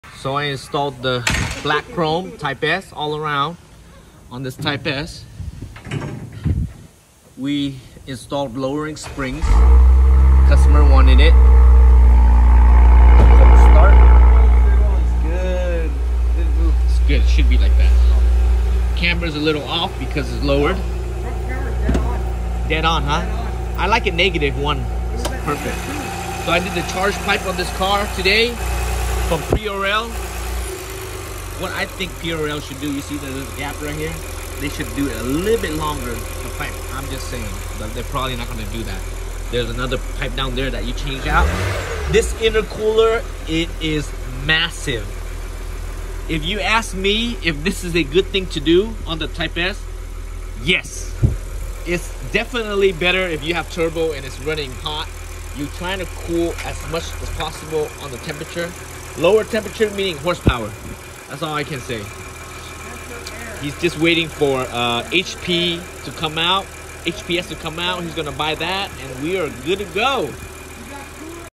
2022 Tlx type s prl inner cooler charge pipe kit
Installed lowering springs I forgot what brand is called. Also did PRL charge pipe kit, black chrome type s emblems. ￼